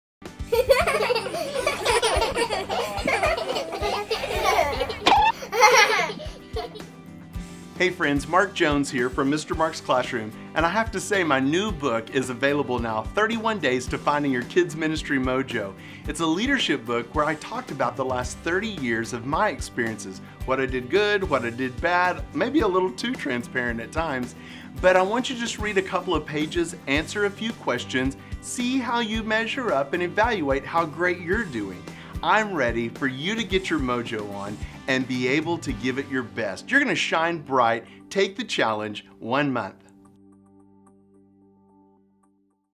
31 Days to Finding your Kids' Ministry Mojo - Part 33
This new book is filled with practical suggestions from Mr. Mark's experience in ministry.  You will be informed and encouraged in your leadership each day for 31 days. It’s time to find your Kids’ Ministry Mojo, so you can be refreshed and shine as you lead. Take the Challenge!